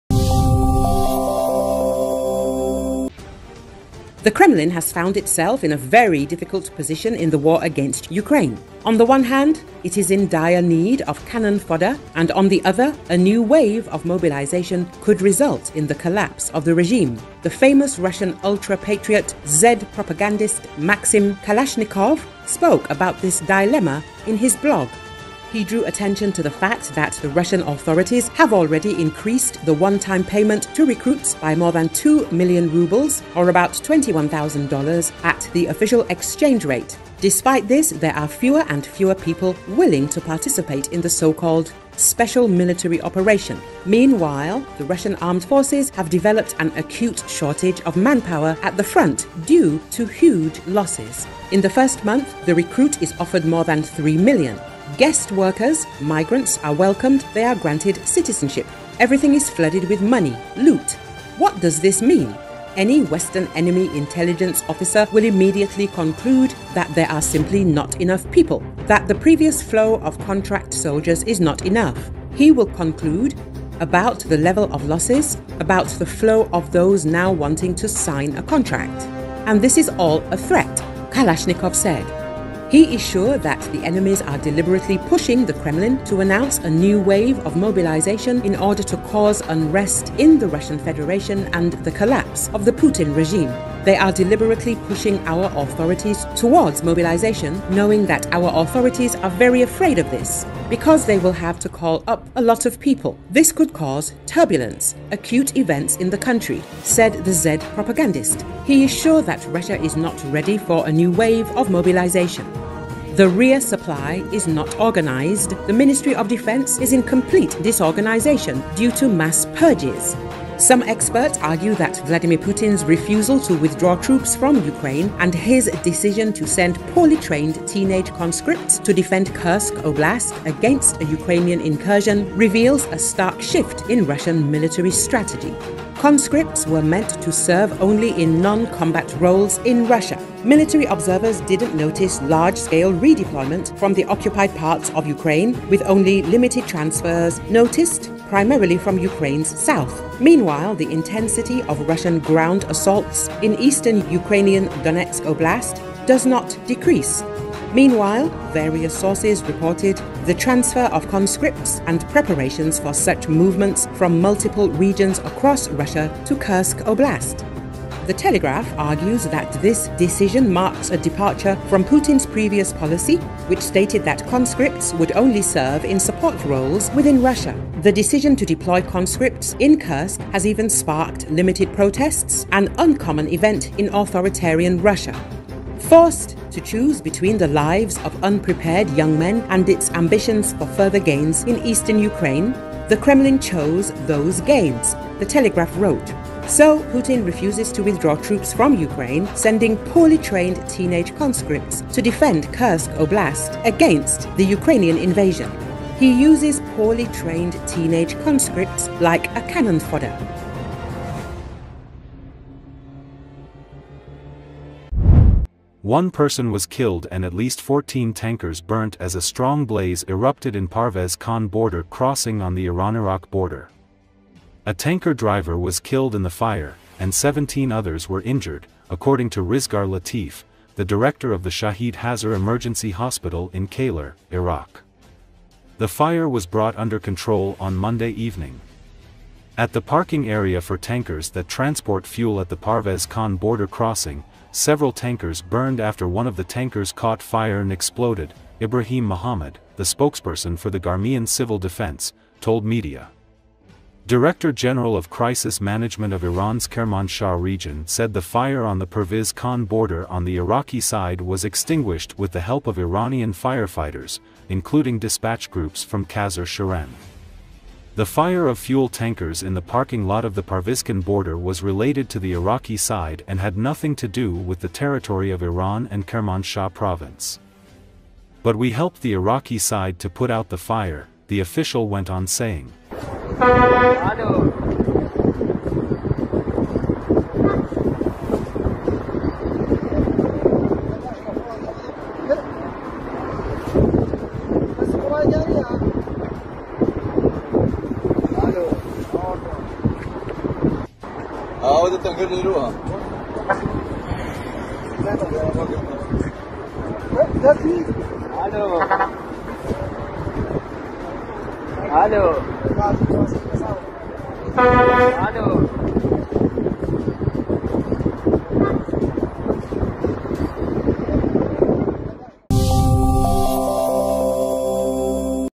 New borders will lead to the collapse of Putin regime in Russia - Russians do not want to fight
The Kremlin has found itself in a very difficult position in the war against Ukraine. On the one hand, it is in dire need of "cannon fodder", and on the other, a new wave of mobilization could result in the collapse of the regime. The famous Russian ultra-patriot, Z-propagandist Maxim Kalashnikov, spoke about this dilemma in his blog.

He drew attention to the fact that the Russian authorities have already increased the one-time payment to recruits by more than 2 million rubles or about $21,000 at the official exchange rate. Despite this, there are fewer and fewer people willing to participate in the so-called "sprcial military operation". Meanwhile, the Russian Armed Forces have developed an acute shortage of manpower at the front due to huge losses.

" In the first month, the recruit is offered more than 3 million. Guest workers, migrants are welcomed, they are granted citizenship... Everything is flooded with money, loot. What does this mean? Any Western enemy intelligence officer will immediately conclude that there are simply not enough people! That the previous flow of contract soldiers is not enough. He will conclude about the level of losses, about the flow of those now wanting to sign a contract... And this is all a threat! " - Kalashnikov said. He is sure that the enemies are deliberately pushing the Kremlin to announce a new wave of mobilization in order to cause unrest in the Russian Federation and the collapse of the Putin regime. " They are deliberately pushing our authorities towards mobilization, knowing that our authorities are very afraid of this. Because they will have to call up a lot of people. This could cause "turbulence", acute events in the country ," - said the Z-propagandist. He is sure that Russia is not ready for a new wave of mobilization. The rear supply is not organized, the Ministry of Defense is in "complete disorganization" due to mass purges.

Some experts argues that Vladimir Putin’s refusal to withdraw troops from Ukraine and his decision to send poorly trained teenage conscripts to defend Kursk Oblast against a Ukrainian incursion reveals a stark shift in Russian military strategy. Conscripts were meant to serve only in non-combat roles in Russia. Military observers didn’t notice large-scale redeployment from the occupied parts of Ukraine, with only limited transfers noticed, primarily from Ukraine’s south. Meanwhile, the intensity of Russian ground assault in eastern Ukrainian Donetsk Oblast does not decrease. Meanwhile, various sources reported the transfer of conscripts and preparations for such movements from multiple regions across Russia to Kursk Oblast.

The Telegraph argues that this decision marks a departure from Putin’s previous policy, which stated that conscripts would only serve in support roles within Russia. The decision to deploy conscripts in Kursk has even sparked limited protests, an uncommon event in authoritarian Russia.

“Forced to choose between the lives of unprepared young men and its ambitions for further gains in eastern Ukraine, the Kremlin chose those gains,” The Telegraph wrote. So, Putin refuses to withdraw troops from Ukraine, sending poorly trained teenage conscripts to defend Kursk Oblast against Ukrainian invasion. He uses poorly trained teenage conscripts like a "cannon fodder"